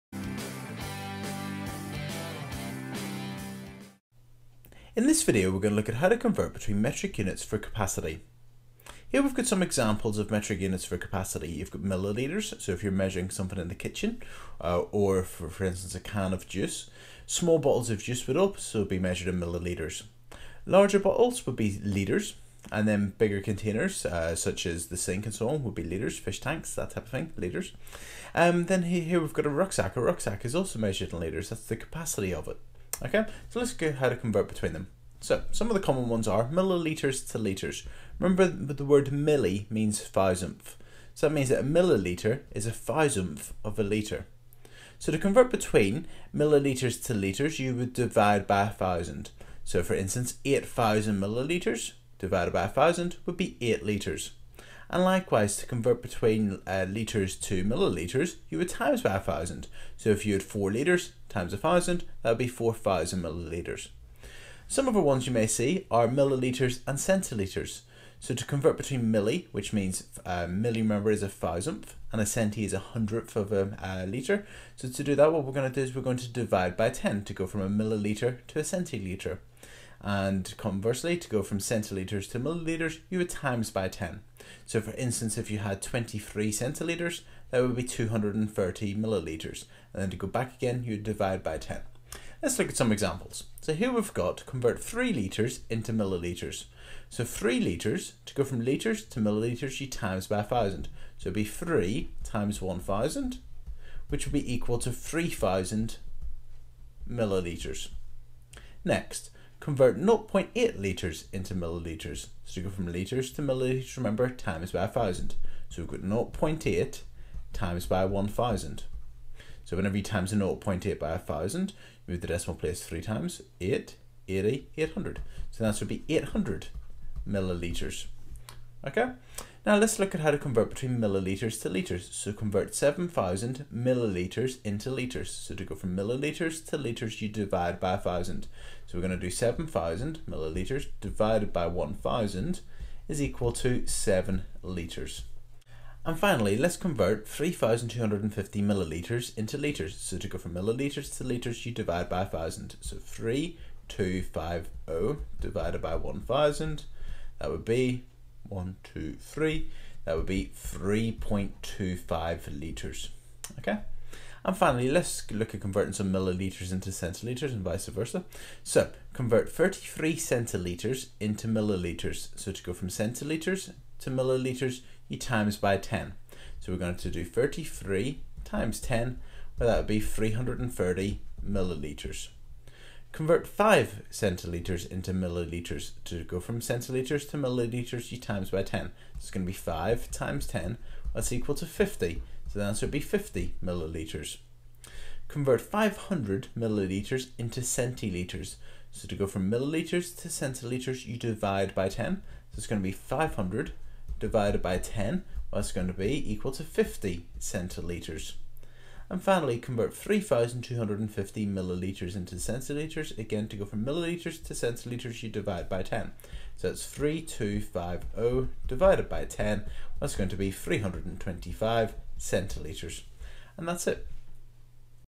Converting Metric Units for Capacity
Corbettmaths - This video explains how to convert between metric units for capacity/volume. For instance millilitres, centilitres and litres.